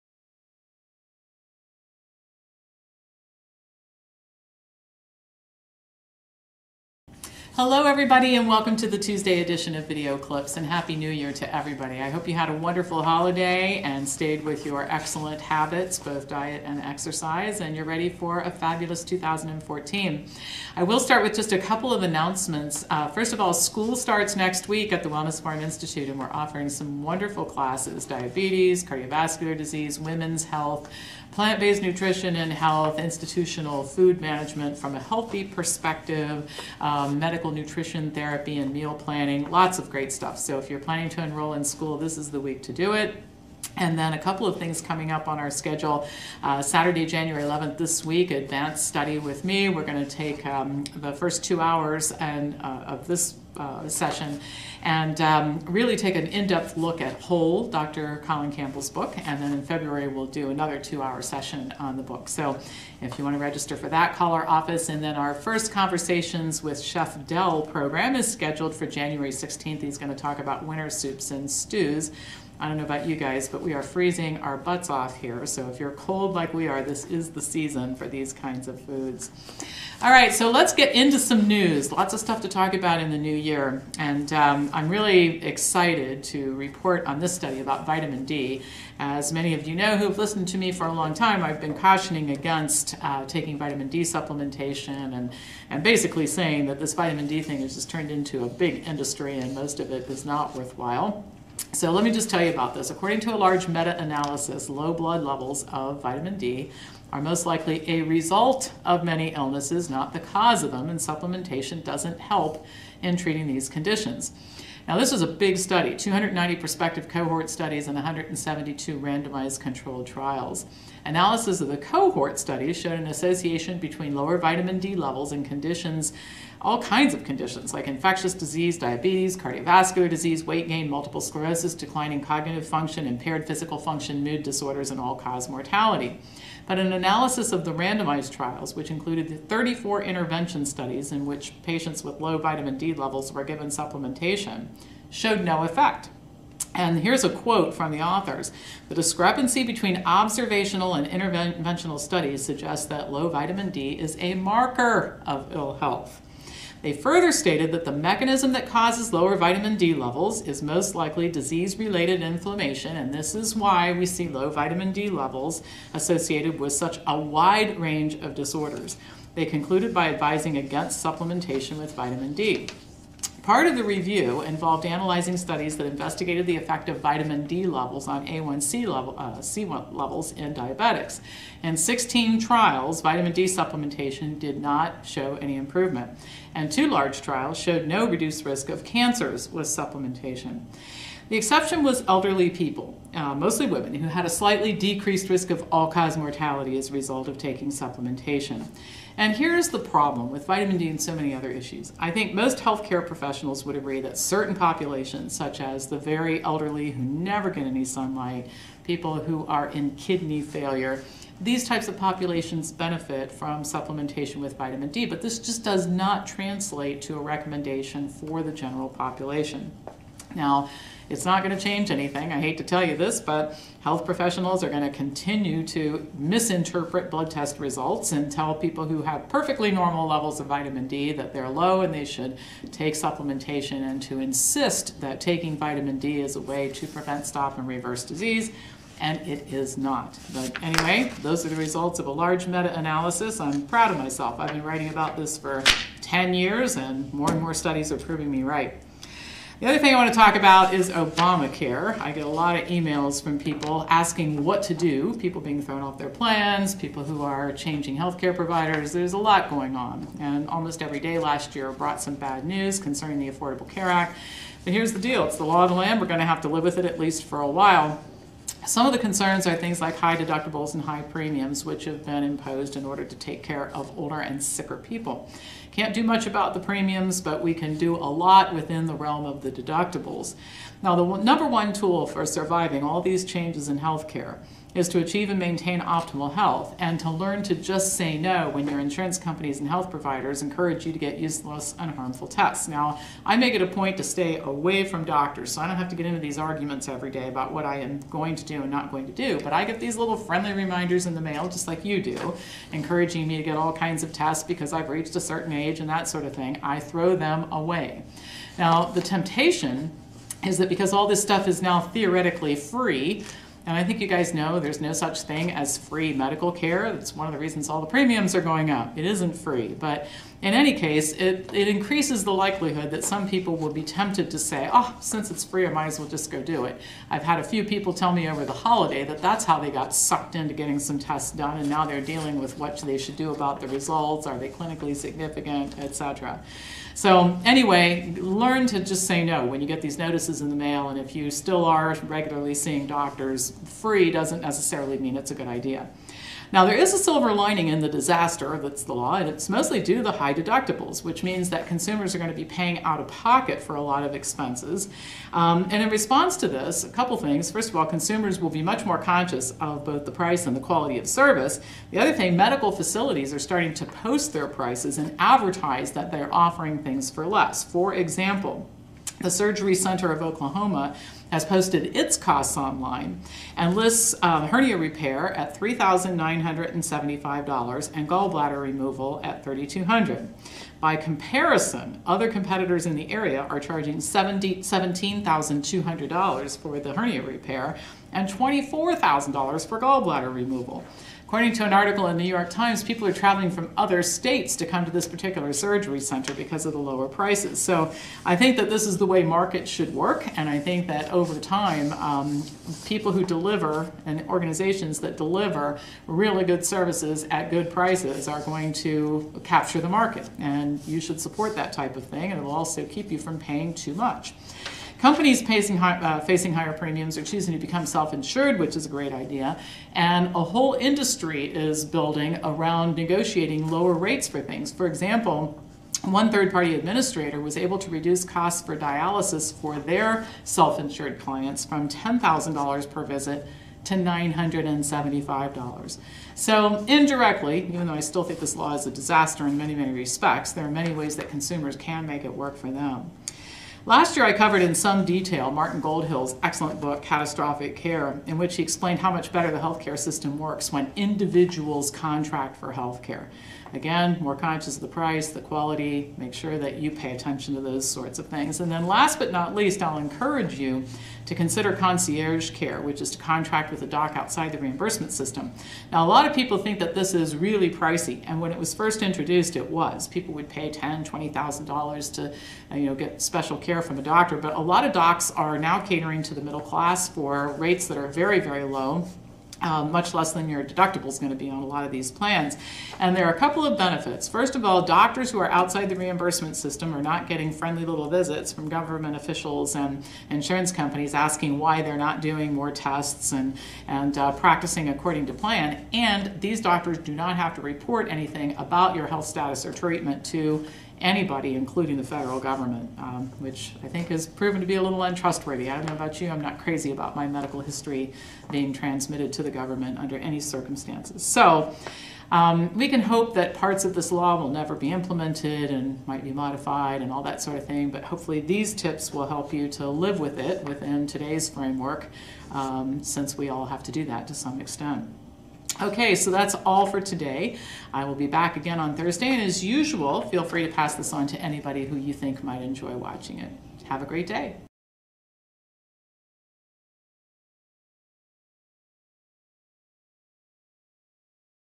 Dr Pam Popper: Vitamin D; Surviving Obamacare
According to a recent meta-analysis of hundreds of studies, low vitamin D levels are a result of many diseases, not the cause of these conditions.

And I'll also offer some tips on how you can survive changes in healthcare due to the Affordable Care Act -- there are a few silver linings in this mess!